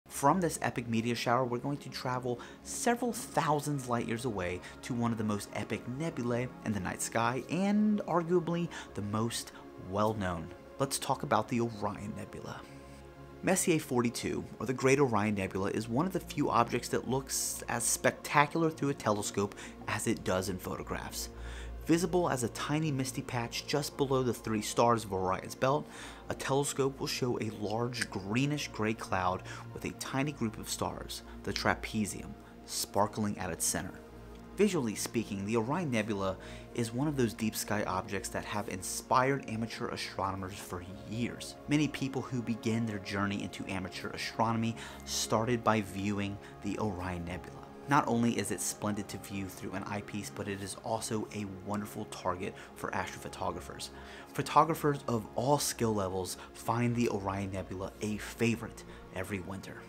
Orion Nebula | December 2025 | High Point Scientific
M42, the Great Orion Nebula, is one of the few objects that looks as spectacular through a telescope as it does in photographs.